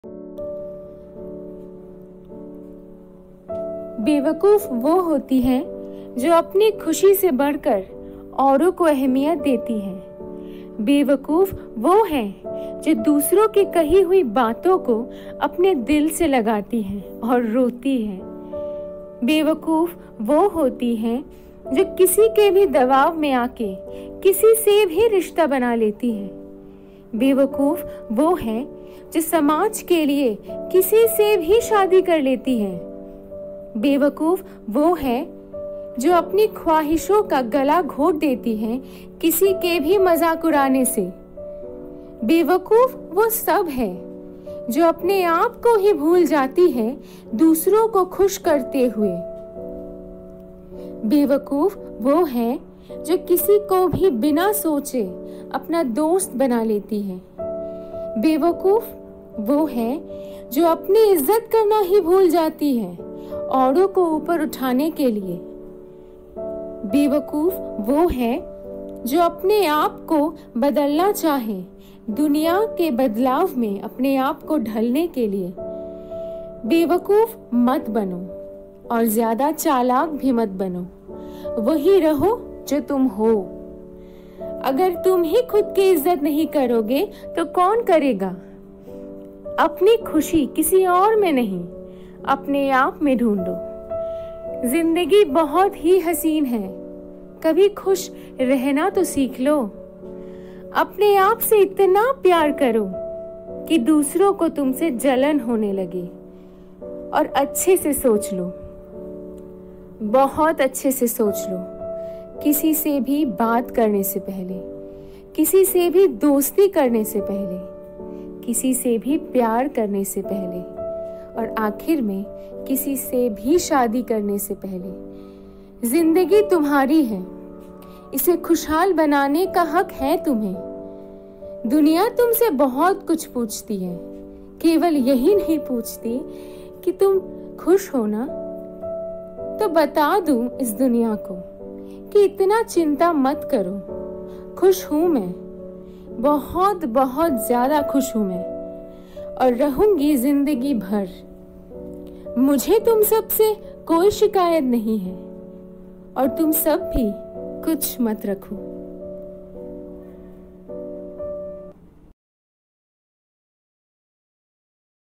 Bewakoof | Self Written | Hindi Poetry | Recitation | Personal Diary | PD
Hello everyone. I just tried to
express my feelings through words. I hope everyone likes this one.
Click on the bell icon for more updates.
A poem written by: Dr.Srija Goswami
Recitation done by: Dr.Srija Goswami
Poem Name: Bewakoof
Bewakoof wo hoti hai jo, apni khushi se badhkar auro ko ahmiyat deti hai;
Bewakoof wo hai jo, dusro ki kahi hui baaton ko apni dil se lagati hai aur roti hai;
Bewakoof wo hoti hai jo, kisi ke bhi dabab me aake kisise bhi rishta baana leti hai;
Bewakoof wo hai jo, samaj ke liye kisi se bhi shaadi kar leti hai;
Bewakoof wo hai jo, apni khwahishon ka gala ghot deti hai, kisi ke bhi mazak urane se;
Bewakoof wo sab hai jo, apne aap ko hi bhul jaati hai, dusro ko khush karte hue;
Bewakoof wo hai jo, kisi ko bhi bina soche, apna dost bana leti hai;
Bewakoof wo hai jo, apni izzat karna hi bhool jaati hai, auro ko upar uthane ke liye;
Bewakoof wo hai jo, apne aap ko badalna chahe, duniya ke badlav me apne aap ko dhalne ke liye;
Bewakoof mat baano, aur zyada chalak bhi mat baano;
Wohi raho, jo tum ho;
Agar tum hi khud ki izzat nahi karoge, to kaun karega?
Aapni khushi kisi aur me nahi, aapne aap me dhundo;
Zindegi bahut hi haseen hai, kabhi khush rehna to seekhlo;
Aapne aap se itna pyaar karo, ki dusro ko tumse jalan hone lage;
Aur acche se soch lo, bahut acche se soch lo;
Kisi se bhi baat karne se pehle;
Kisi se bhi dosti karne se pehle;
Kisi se bhi pyaar karne se pehle;
Aur akhir me, kisi se bhi shaadi karne se pehle;
Zindegi tumhari hai, ise khushaal banane ka haq hai tumhe;
Duniya tumse bahut kuch poochti hai, kewal yahi nahi poochti ki, tum khush ho na?
To bataan du is duniya ko, ki itna chinta mat karo;
Khush hun main,bahut, bahut zyada khush hun main;
Aur rahungi zindegi bhar;
Mujhe tum sab se koi shikayat nahi hai;
Aur tum sab bhi kuch mat rakho.
                               - Dr.Srija Goswami